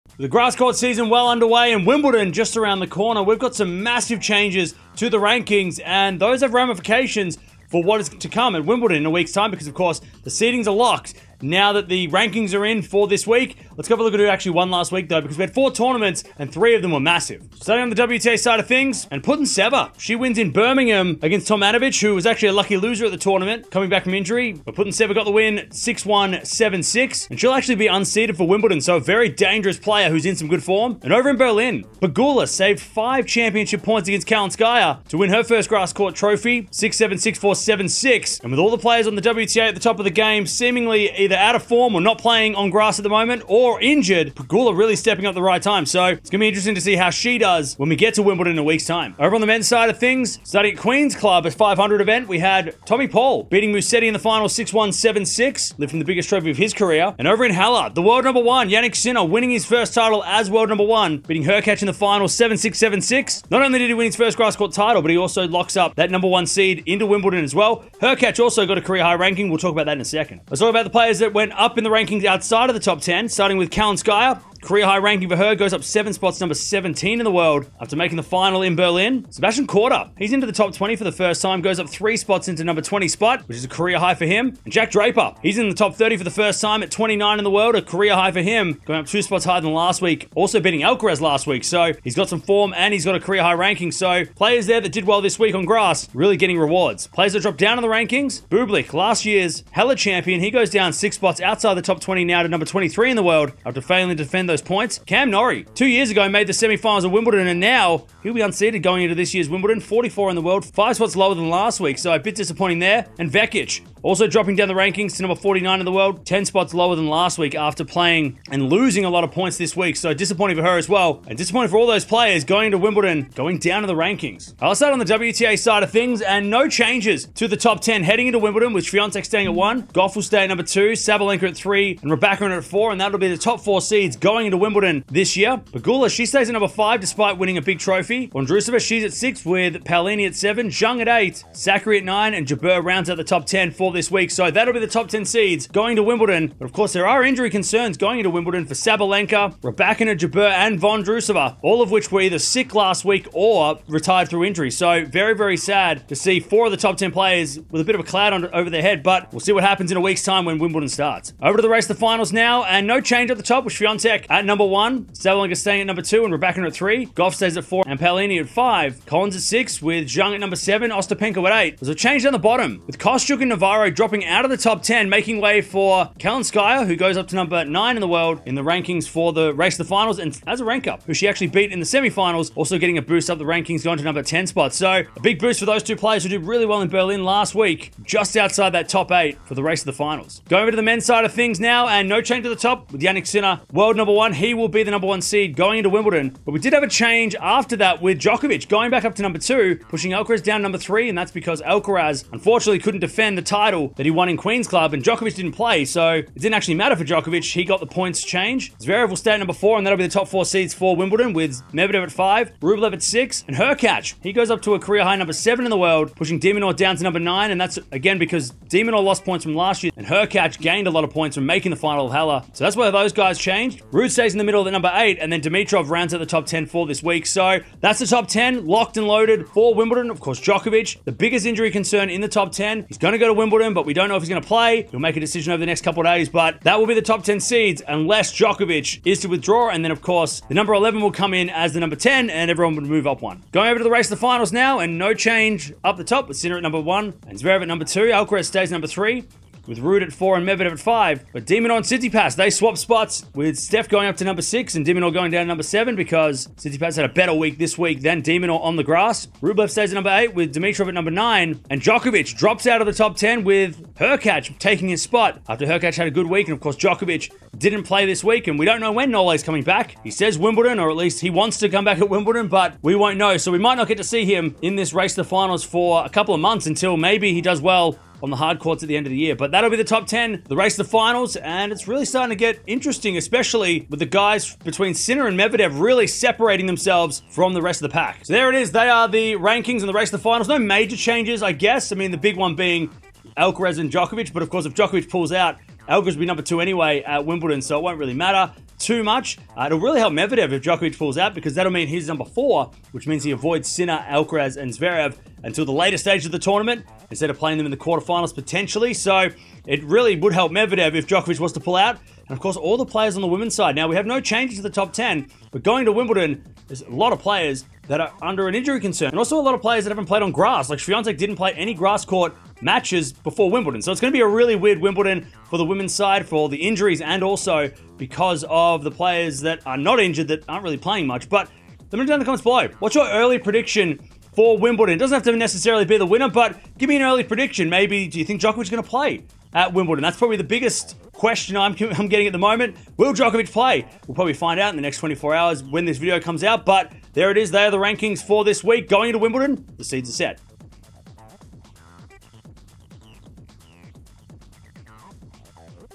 Sinner, Pegula Win Titles | Seeds Locked In for Wimbledon 2024 | Tennis Rankings
The Wimbledon 2024 seedings are set with Sinner, Djokovic and Alcaraz the Top 3 seeds for the men and Swiatek, Gauff and Sabalenka for the ladies. Sinner also won a title this week in Halle and Pegula won over in Berlin.

0:00 | Intro
0:17 | Past Results
1:10 | Rise & Fall
2:04 | WTA Rankings
2:41 | WTA Finals Race
3:10 | ATP Rankings
4:03 | ATP Finals Race
4:47 | Outro